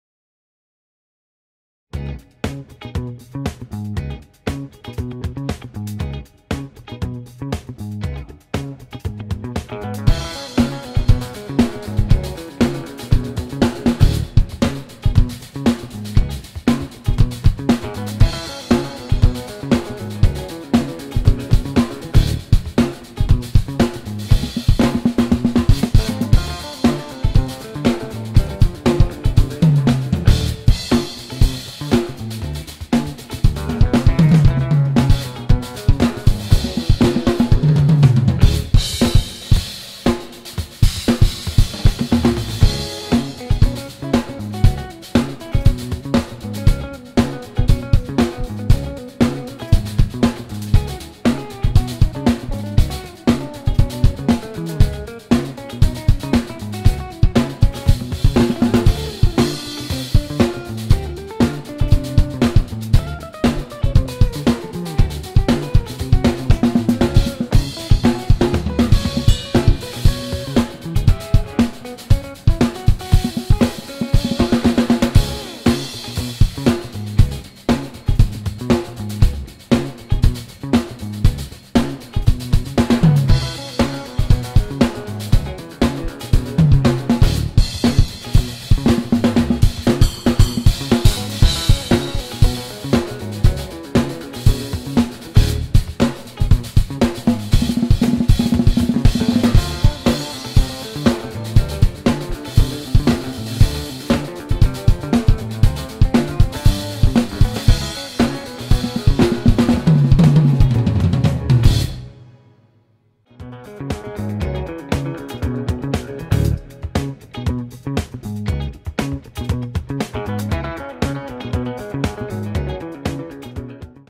16beat Funky Drumming - Two handed Hi-hat (양손으로 하이햇 연주하기)
Hey Mr. Funky Drummer

Gear
Tama Starclassic B/B Drum set

Sonor Benny greb signature snare drum
Zildjian A custom 16, K series 18 Crash Cymbal
Zildjian K 20inch  Ride Cymbal
Hi-hat  Zildjian Efx 16 (top) K  Crash 16 (bottom)

Shure Drum Mic & Shure earphone

Follow MainDrummer AdamSeo